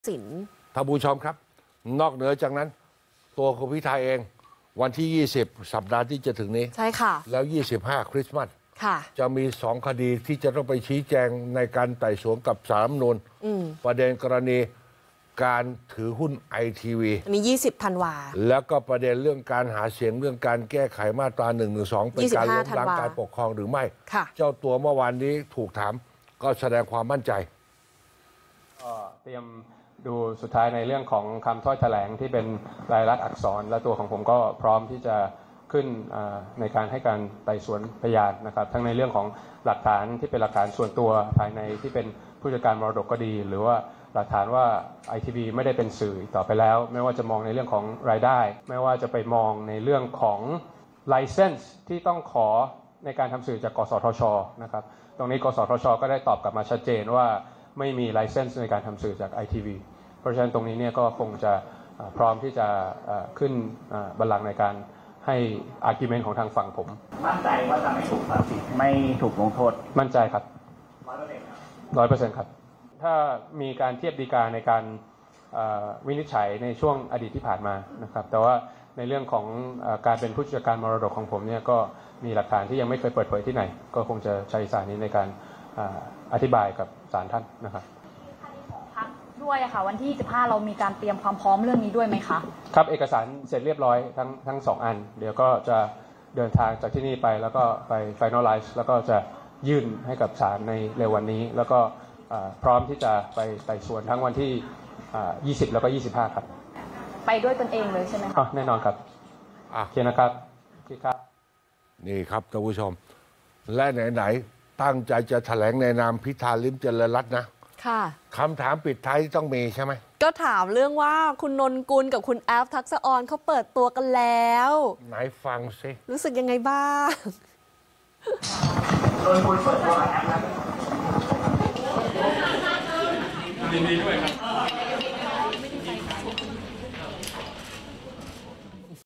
'พิธา' มั่นใจไม่ผิดถือครองหุ้นสื่อ พร้อมไปศาล ฟังคำพิพากษาด้วยตัวเอง 20 ธ.ค.นี้
วานนี้ 15 ธันวาคม 2566 นายพิธา ลิ้มเจริญรัตน์ ส.ส. บัญชีรายชื่อพรรคก้าวไกล กล่าวถึงกรณีที่สัปดาห์หน้าศาลรัฐธรรมนูญนัดพิจารณาคดี ถูกกล่าวหาถือครองหุ้นสื่อในวันที่ 20 ธันวาคมนี้ว่า ได้เตรียมความพร้อมไว้แล้วโดยวันนี้จะดูรายละเอียดสุดท้ายในถ้อยแถลงที่เป็นลายลักษณ์อักษร และพร้อมที่จะขึ้นให้การไต่สวนพยานทั้งในเรื่องของหลักฐานที่เป็นหลักฐานส่วนตัว ในส่วนของผู้จัดการมรดกก็ดี และหลักฐานว่าไอทีวีไม่ได้เป็นสื่อต่อไปแล้ว ไม่ว่าจะมองเรื่องของรายได้ เรื่องของใบอนุญาตที่ต้องขอในการทำสื่อจาก กสทช.

ซึ่งตรงนี้กสทช.ตอบกลับมาชัดเจนว่าไม่มีใบอนุญาตทำสื่อของ itv ดังนั้นตรงนี้ก็พร้อมที่จะขึ้นบัลลังก์ในการให้ปากคำในฝั่งของตนและมั่นใจ 100% ว่าจะไม่ผิด ทั้งนี้แนวเรื่องของการเป็นผู้จัดการมรดกมีหลักฐานที่ไม่เคยเปิดที่ไหนก็จะใช้ส่วนนี้จะอธิบายต่อสาธารณะ

ส่วนคดี นโยบายแก้ไขมาตรา 112 ของพรรคก้าวไกล ที่อาจเข้าข่ายล้มล้างการปกครองในระบอบประชาธิปไตยอันมีพระมหากษัตริย์ทรงเป็นประมุขนั้น นายพิธา กล่าวว่าเอกสารก็เสร็จเรียบร้อยทั้ง 2 ส่วนแล้ว โดยจะตรวจสอบและยื่นให้ศาลในเร็ววันนี้ และพร้อมที่จะได้รับการตรวจสอบทั้งวันที่ 20 และ 25 ธ.ค. และซึ่งตนจะเดินทางไปศาลเพื่อฟังคำวินิจฉัย ด้วยตัวเอง